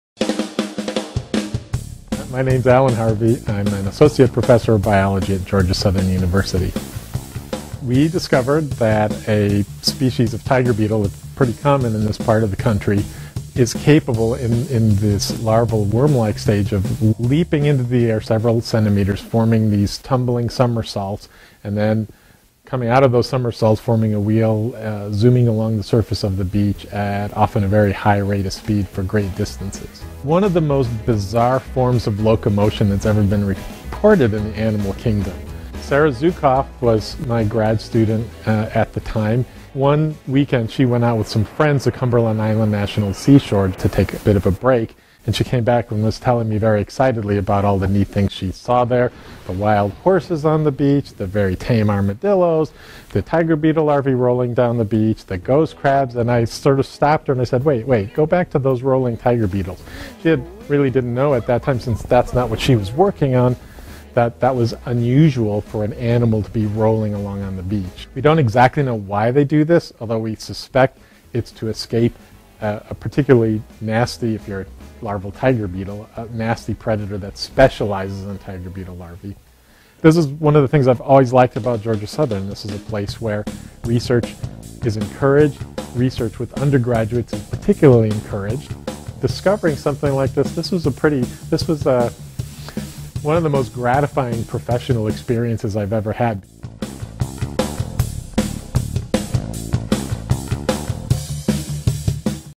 Beetle Larvae Show Wheel Locomotion
Research conducted by Georgia Southern Univ. associate professor of biology Alan Harvey, along with former Georgia Southern Univ. biology graduate student Sarah Zukoff will be published in PLoS ONE. Harvey and Zukoff recently documented the first case of wind-powered wheel locomotion in larvae of "one of the best-studied insects in North America," the Eastern Beach Tiger Beetle Cicindela dorsalis (subspecies media). Source: GSU